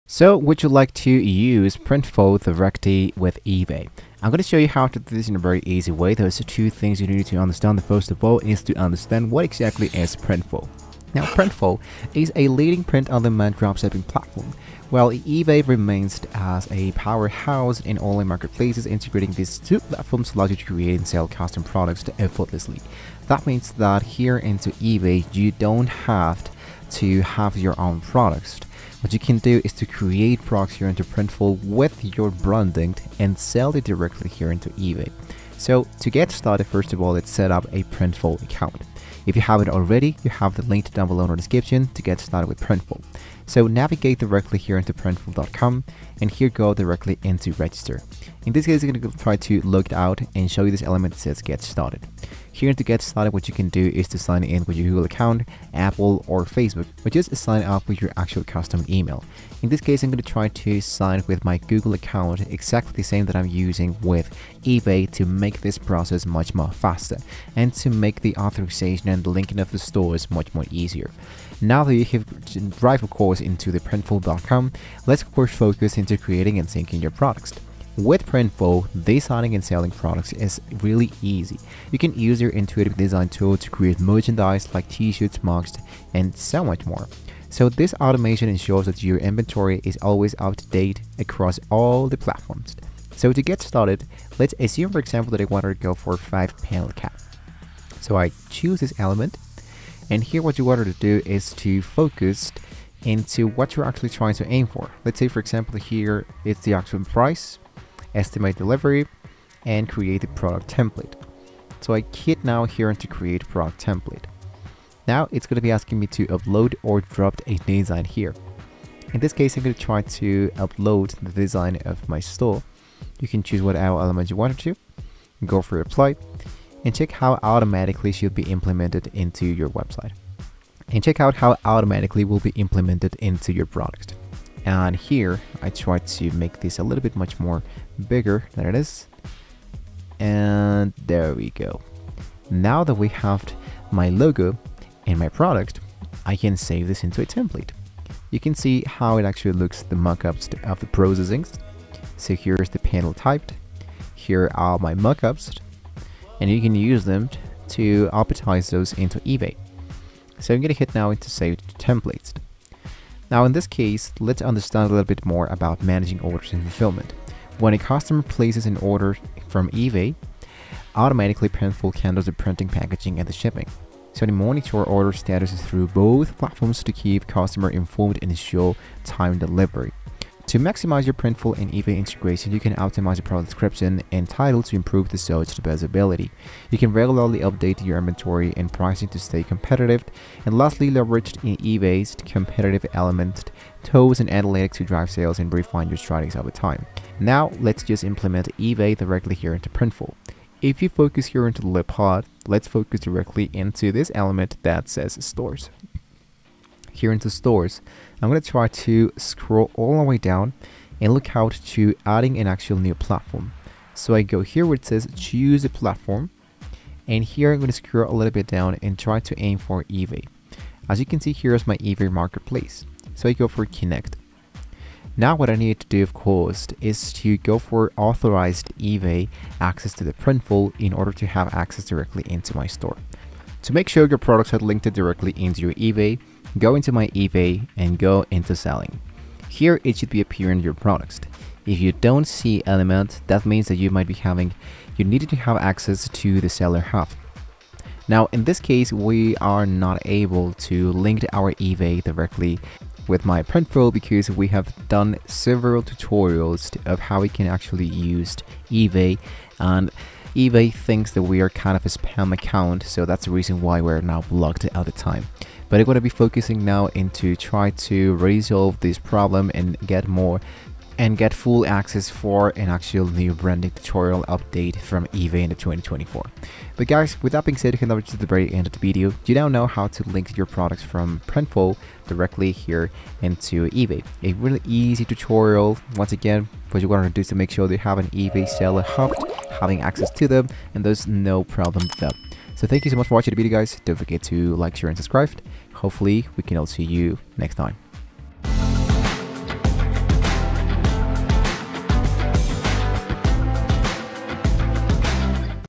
How to Use Printful With Ebay (2026) Step by Step
To use Printful with eBay, start by creating an account on both platforms. Once logged in to Printful, connect your eBay store to Printful by going to the "Stores" tab and selecting "Add store." Follow the prompts to link your eBay account. Next, choose products from Printful's catalog to add to your eBay store. Customize your products with designs, colors, and sizes.

Set pricing and shipping options according to your preferences. When a customer makes a purchase on eBay, Printful will automatically fulfill the order, print the design on the chosen product, and ship it directly to the customer. Monitor your sales and manage your store effortlessly with Printful's integration with eBay.